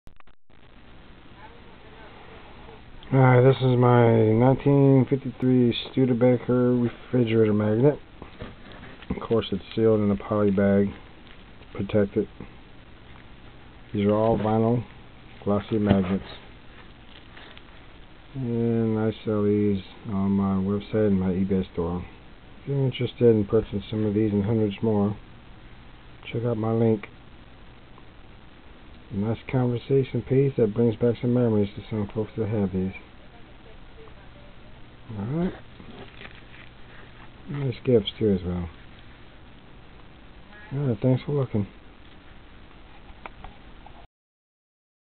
1953 Studebaker Refrigerator Magnet
1953 Studebaker Cool refrigerator magnet. All vinyl and glossy.  Measures 3 3/4" x 5 3/4" Great on a tool box, file cabinet or any smooth surface.   All sealed in polybags for added protection.